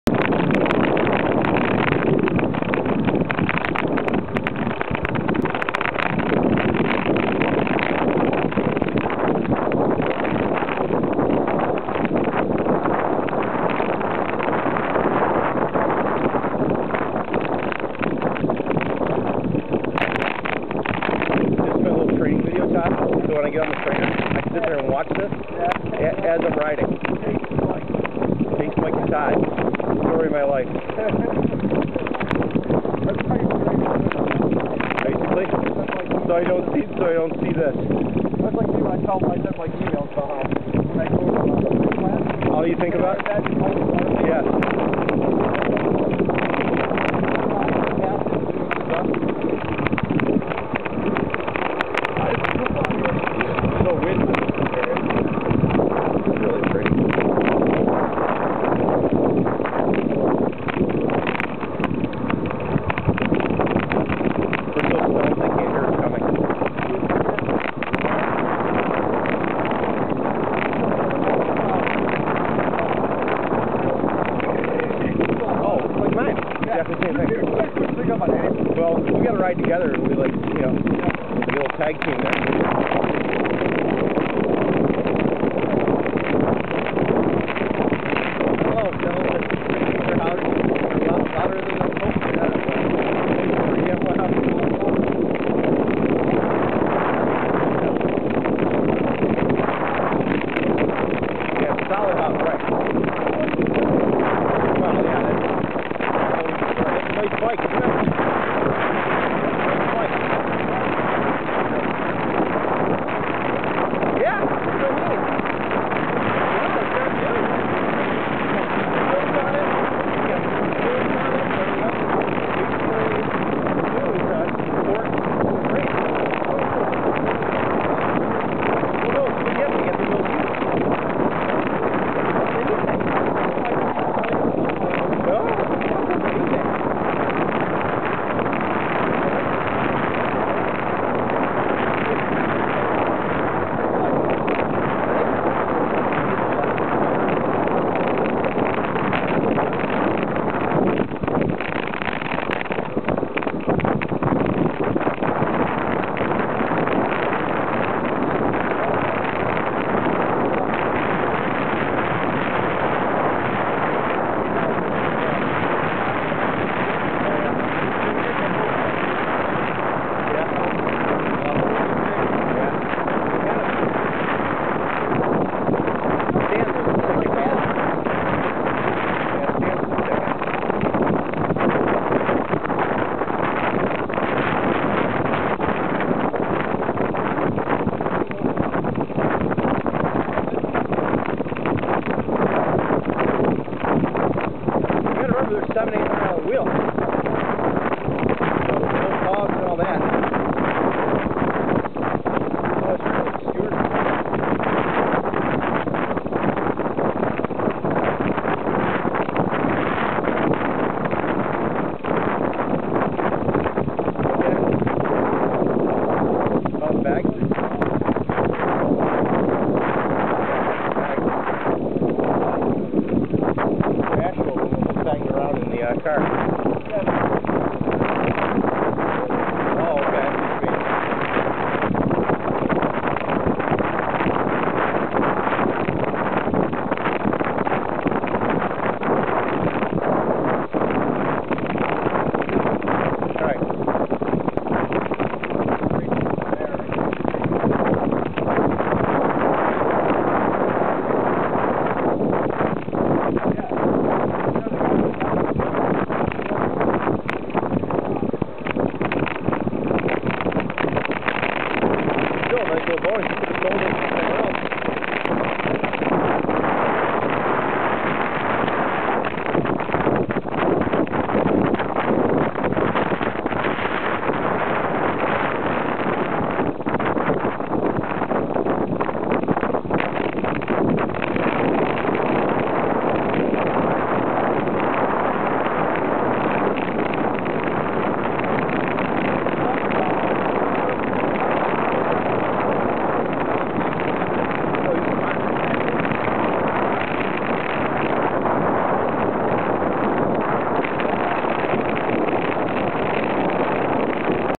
P1030774.MOV
Sunday Fixied Gear Ride :-)
Late November in Buffalo.